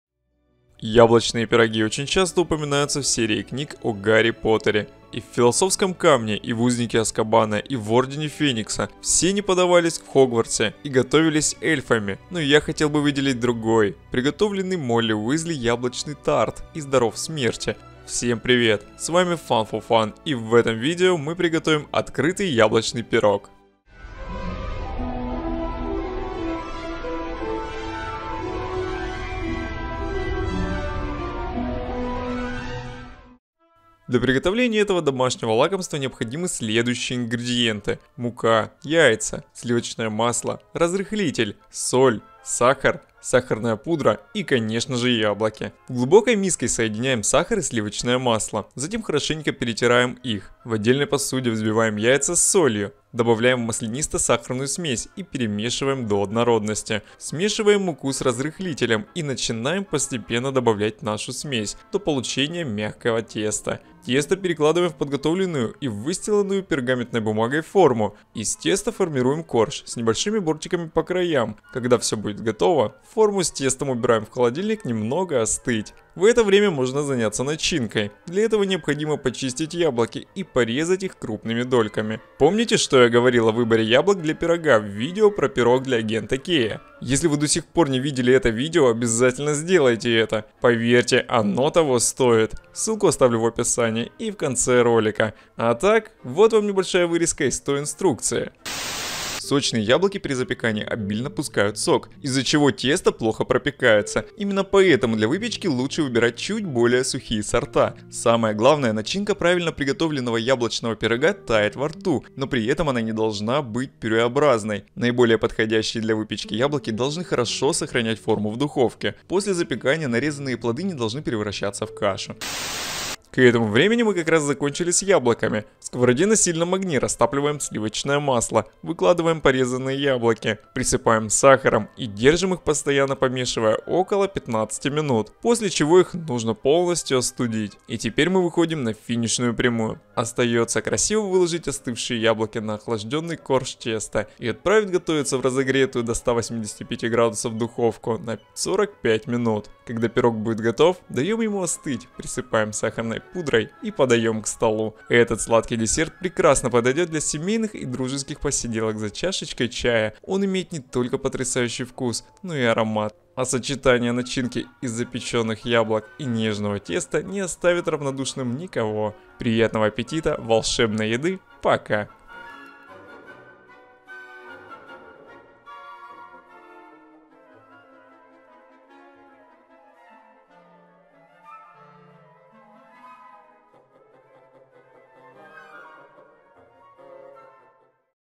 #9 Яблочный пирог миссис Уизли - Harry Potter and the Magic Kitchen - Кухня Гарри Поттера
Яблочные пироги очень часто упоминаются в серии книг о Гарри Поттере, но я хотел бы приготовить именно открытый яблочный пирог миссис Уизли!

Ингредиенты для теста:
• Сахар – 130-150 гр.
• Соль – щепотка

Ингредиенты для начинки:
• Яблоки – 700-800 гр.
• Сливочное масло – 100 гр
• Сахарный песок – 50-65 гр.